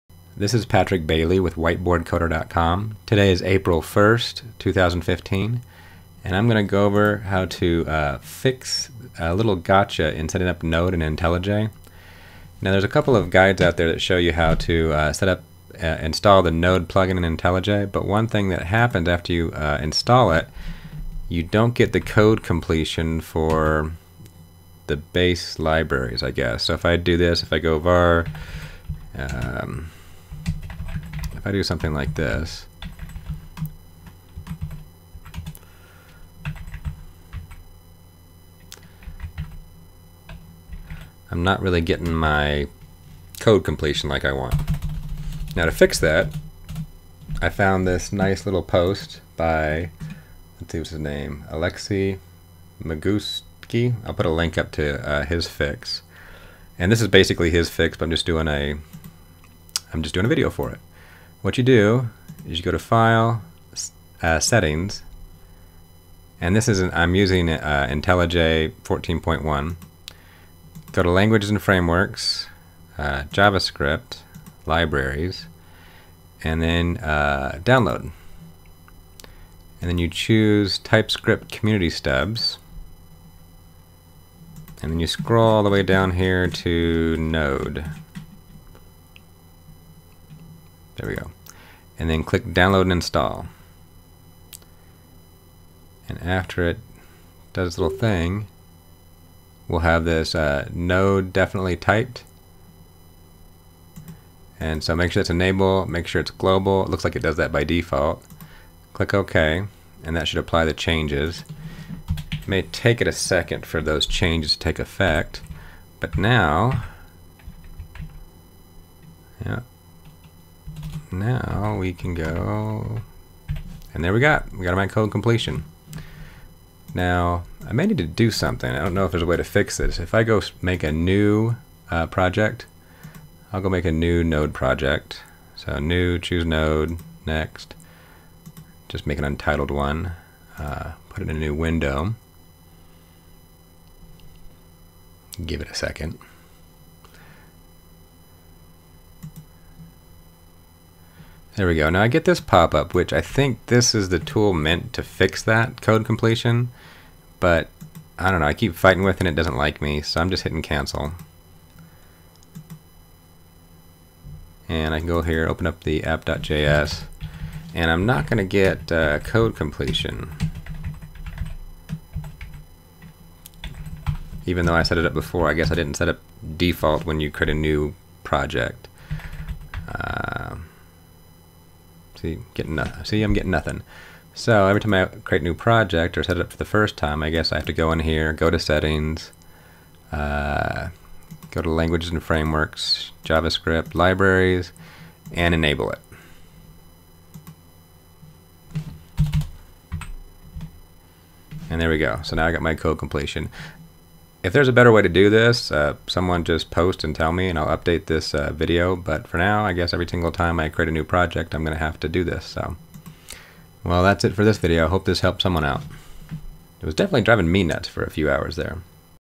Setting up Code Completion for Node Intellij
After installing the Node plugin for Intellij I was having problems getting the code completion to work.  Thanks to the post from Alexey Migutsky